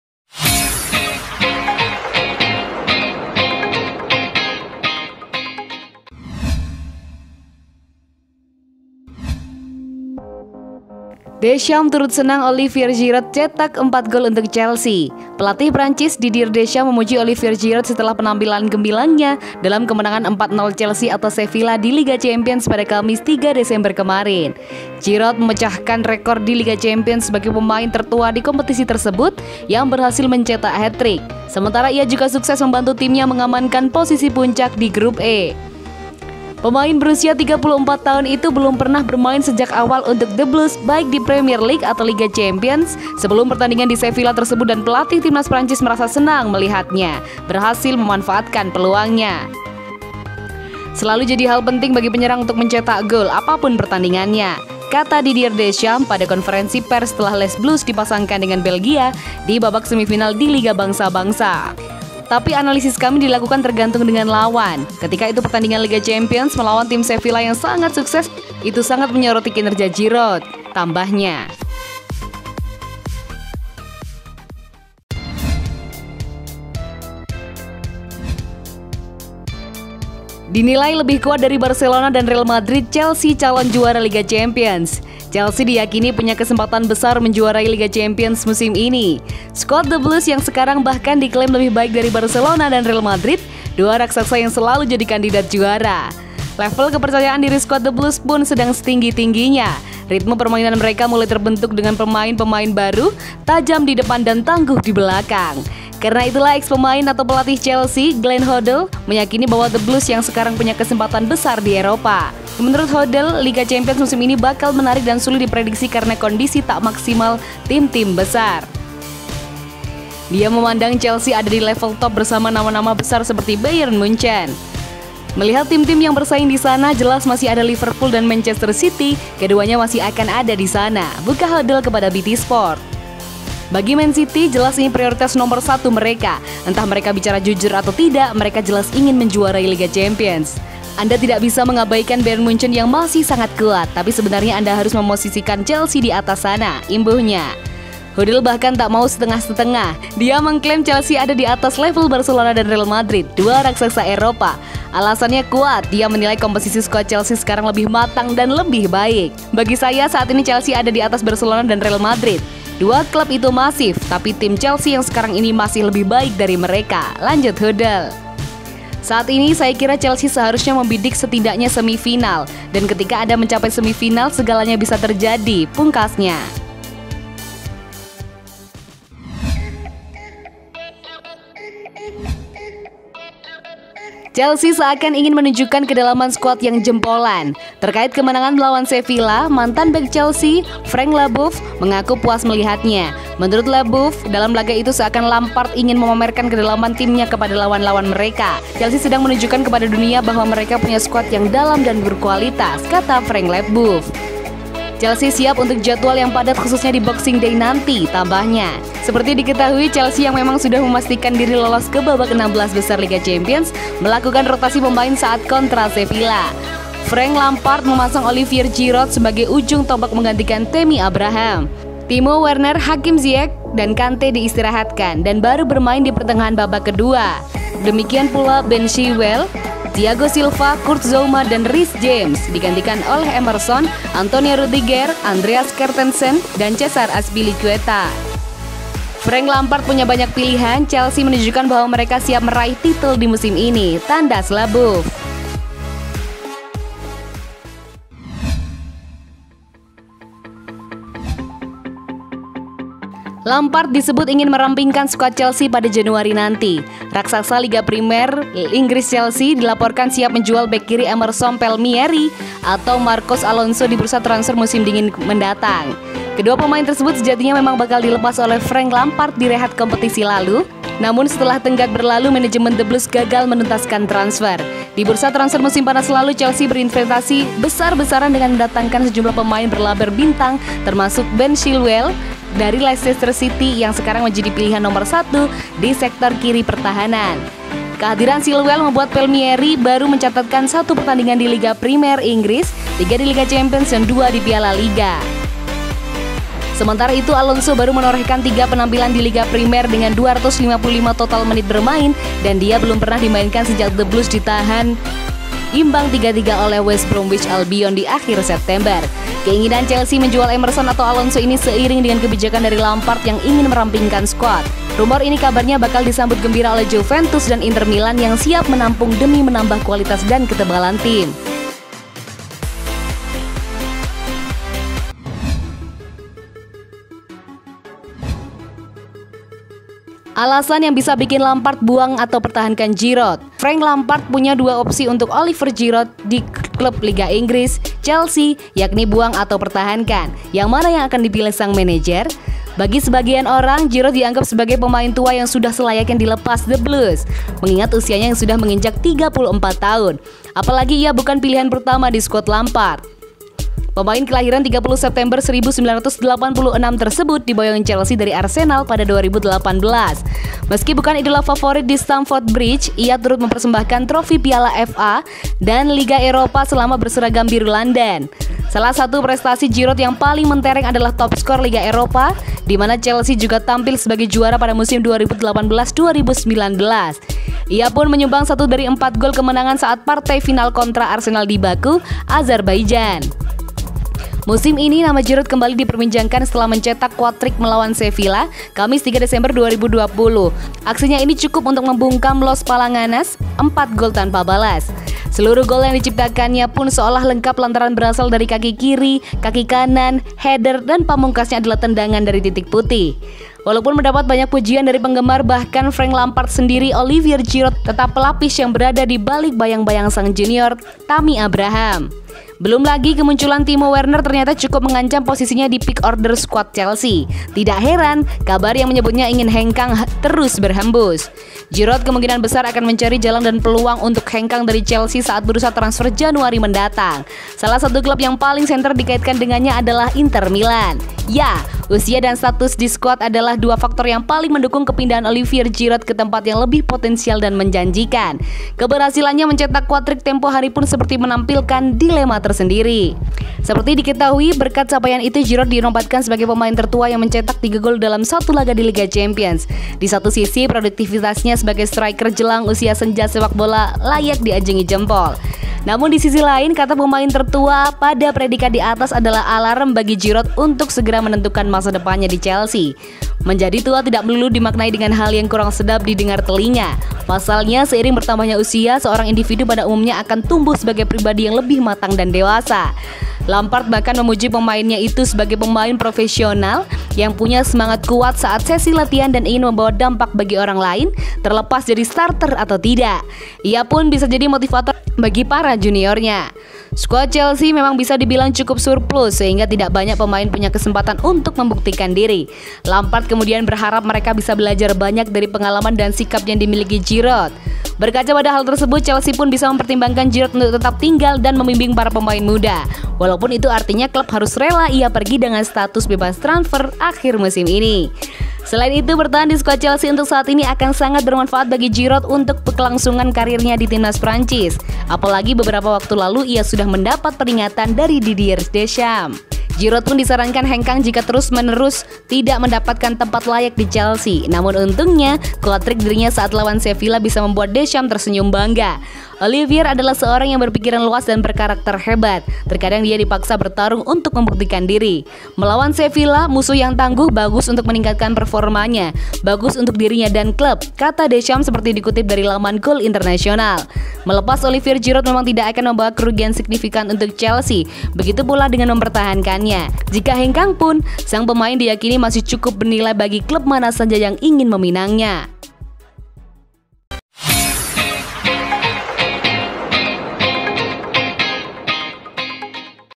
Chelsea Lebih Kuat Dari Barca-Madrid 🔥 Buang / Pertahankan Giroud? 🔵 Kedalaman Skuad 💪 Juara UCL
Selamat datang di channel 🔥Chelsea Fans Club🔥